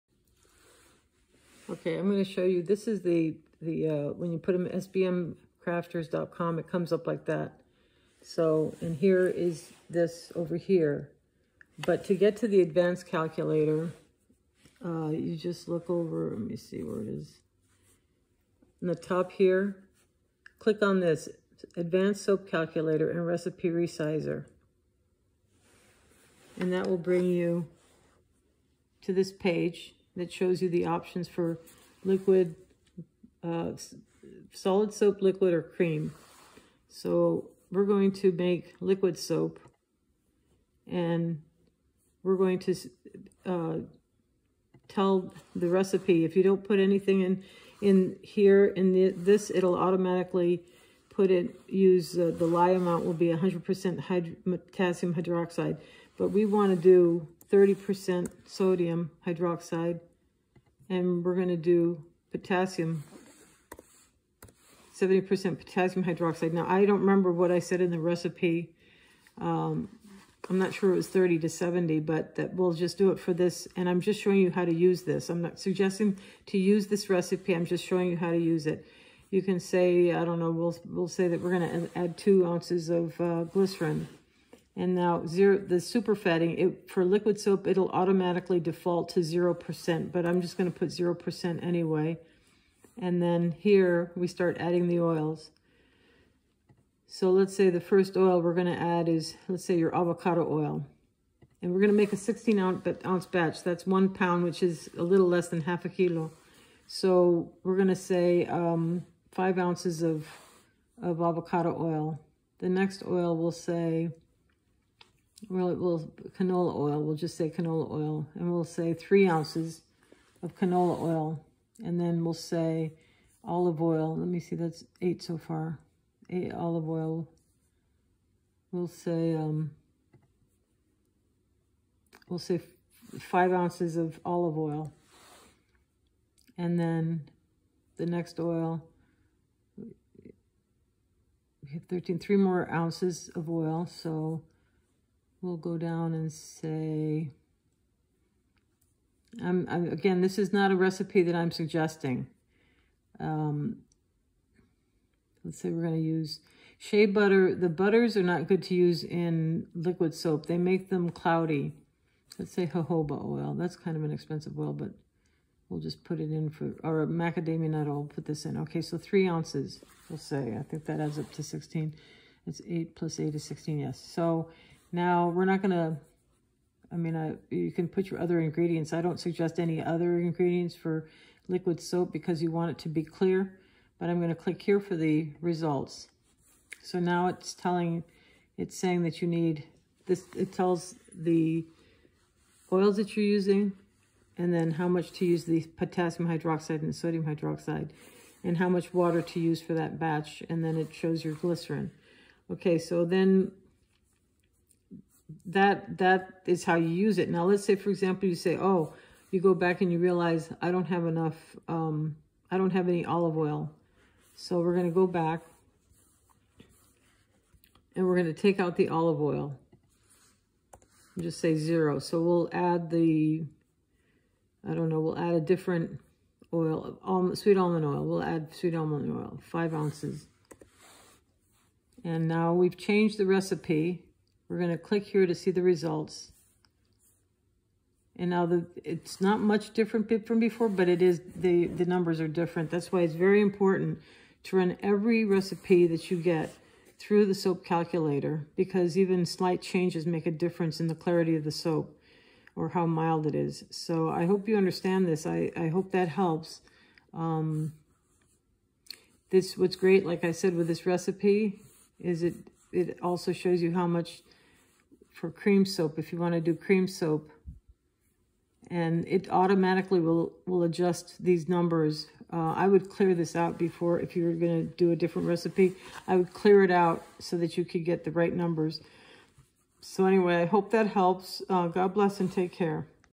How to use an advanced soap calculator to make solid, cream or liquid soap.
This is a short video to show how to use an advanced calculator for making solid, liquid or cream soaps. (Pardon the jiggling--I used my phone to make this quick video to answer questions a couple of people had.)

By the way, the recipe I used did call for 30% sodium hydroxide and 70% potassium hydroxide. This makes a nice, thick liquid soap. You can do 100% potassium hydroxide also. The soap isn't as thick, but is nice.

Please post any questions, comments, feedback, etc. below.